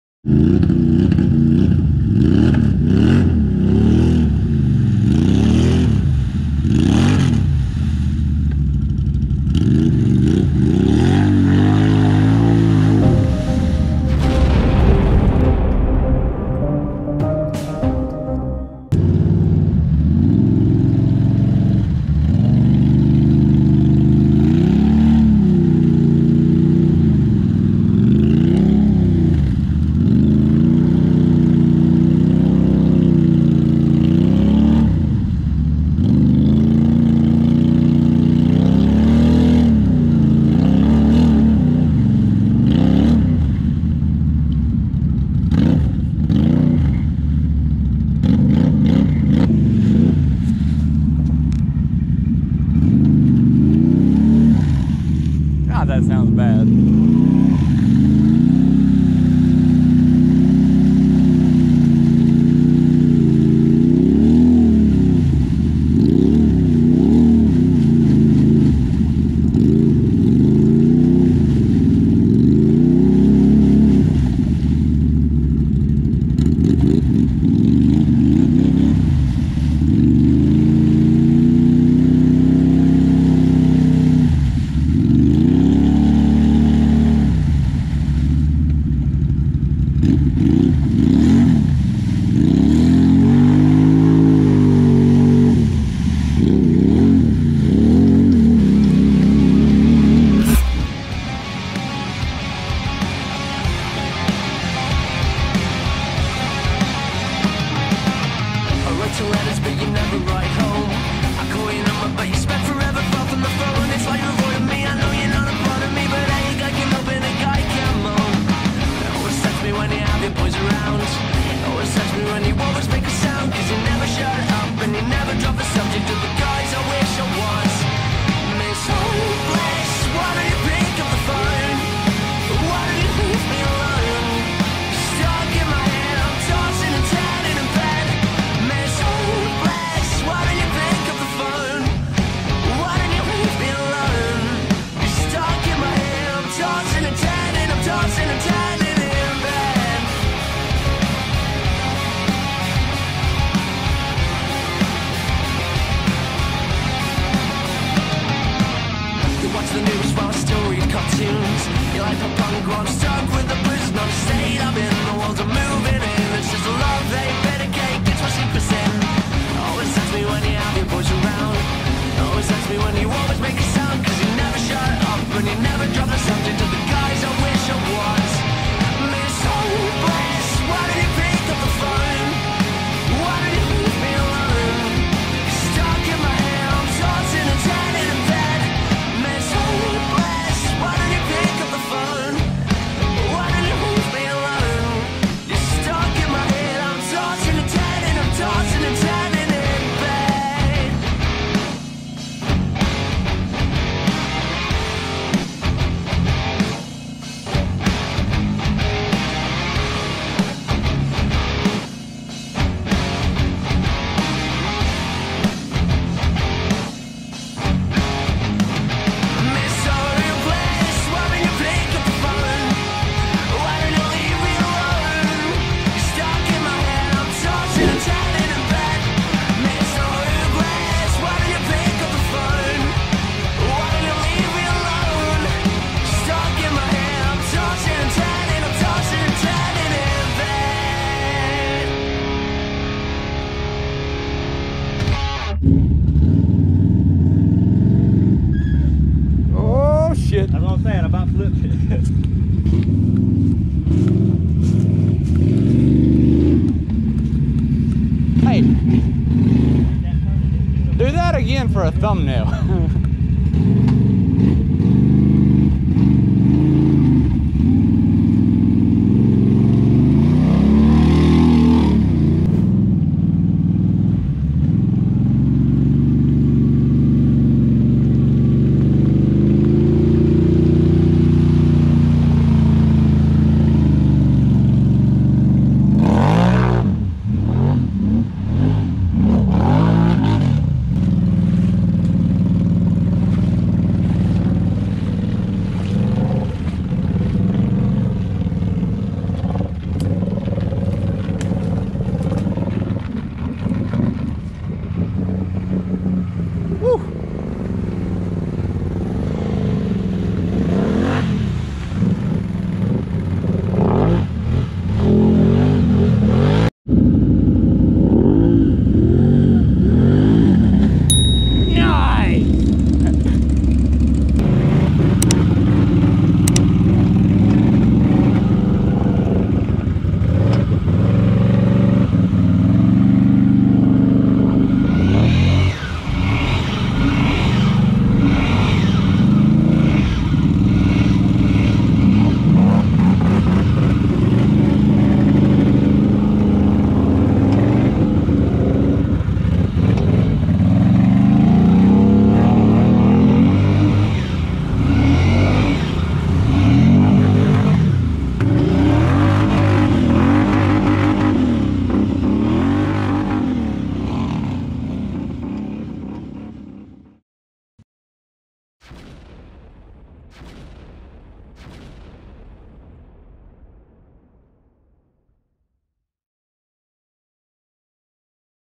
CAN AMS AND GOPROS (Testing GoPro 4k footage)
Sloppy edit just testing the 4k on the GoPro with a few clips from this past weekend.